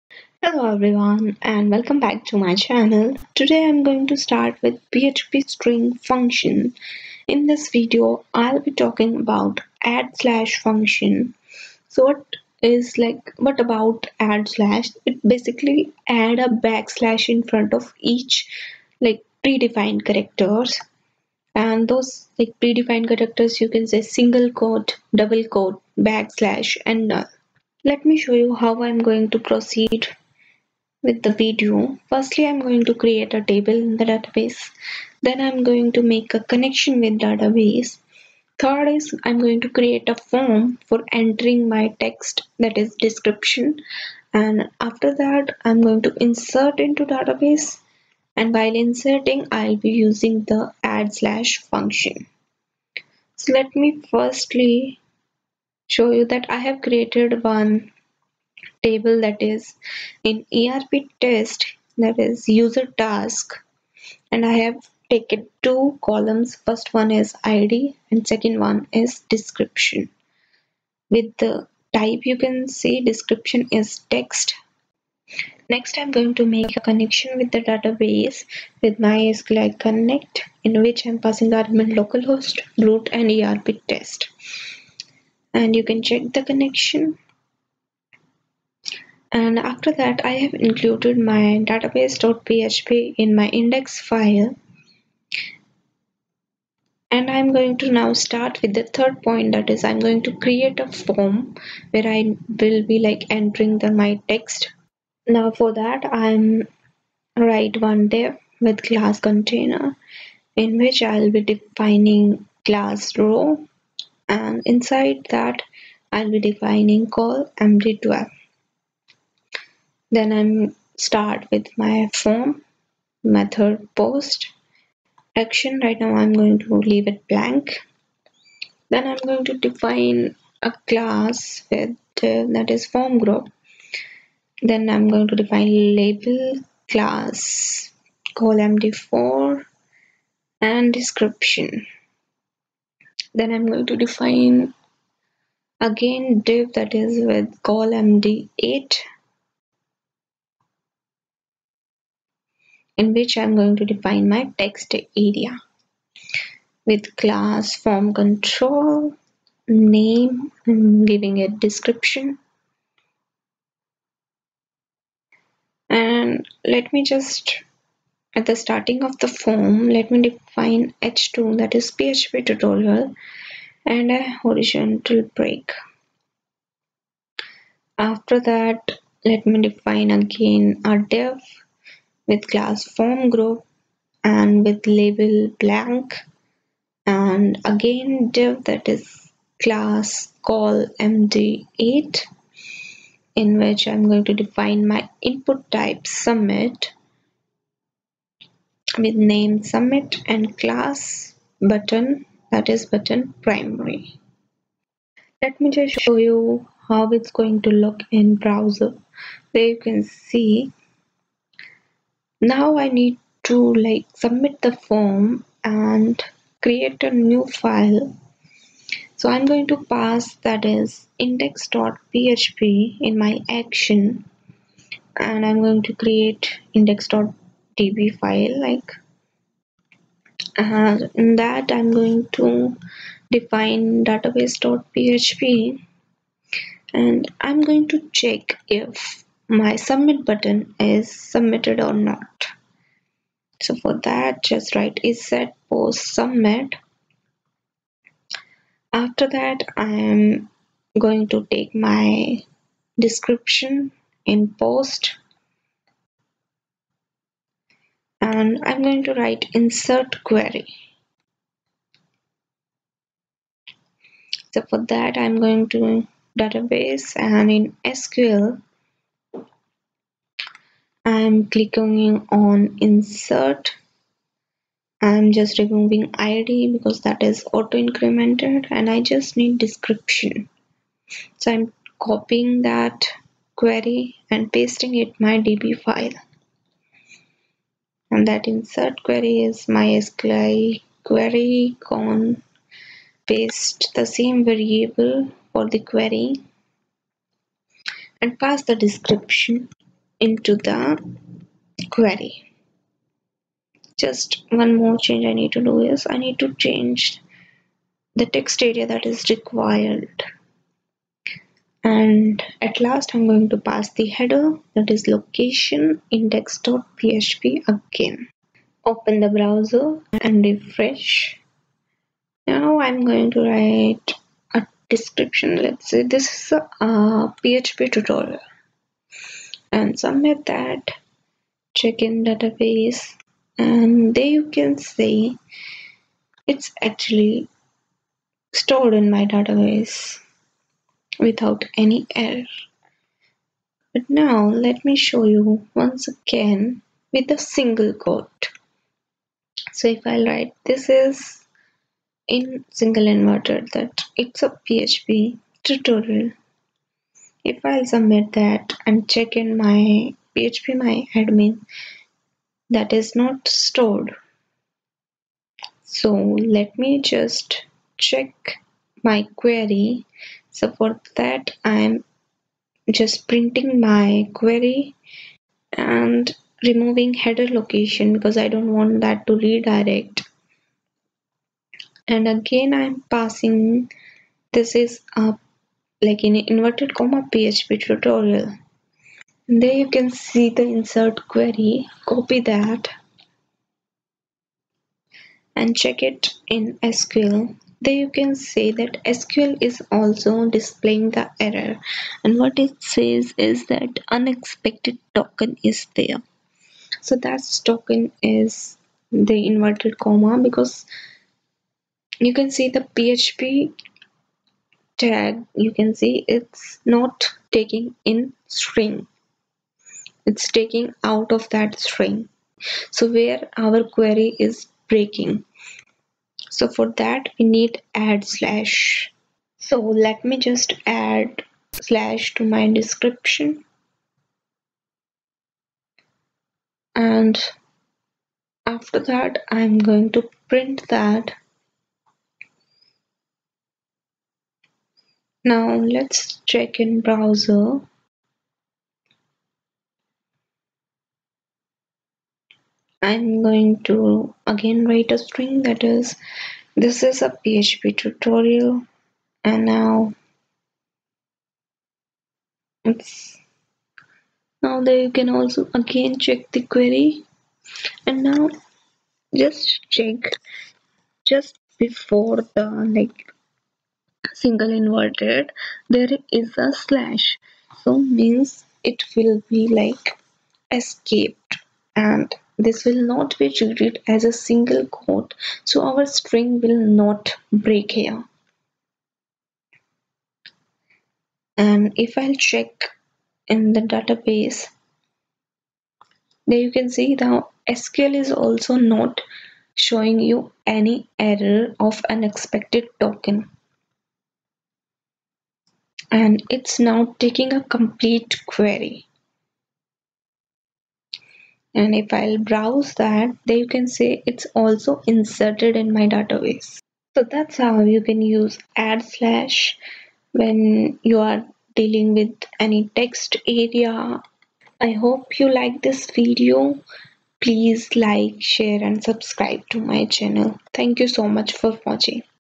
String Function in PHP - addslashes | Knowledge Thrusters
This php video tutorial is about how you can use addslashes string function while inserting the data into database.

addslashes  - add a backslash in front of predefined characters.
Steps:
Create a table
Make a connection with database mysql
Create a form for description
 database file insert into database file using addslashes function.